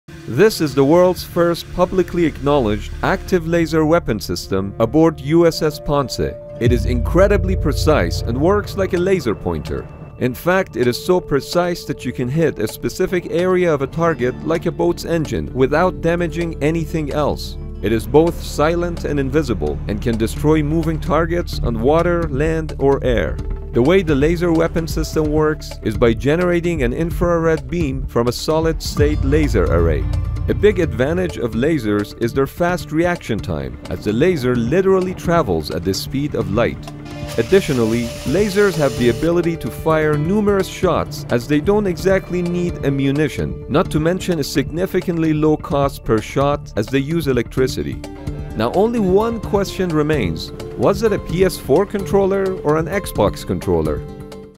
Laser Weapon that you can't See or Hear
This is a laser weapon system developed by the US Navy and being tested on USS Ponce.